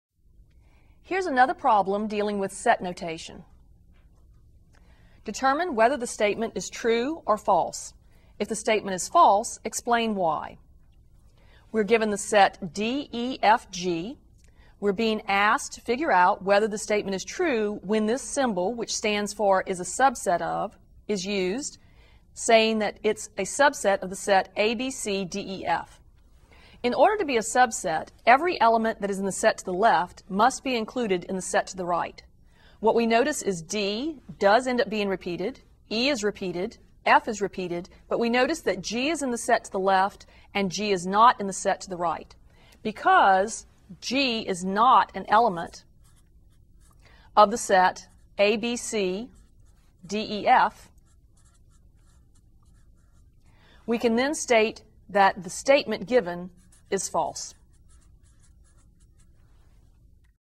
Blitzer Thinking Mathematically Ch 2 Ex 5
A professor solves a true/false problem using set notation.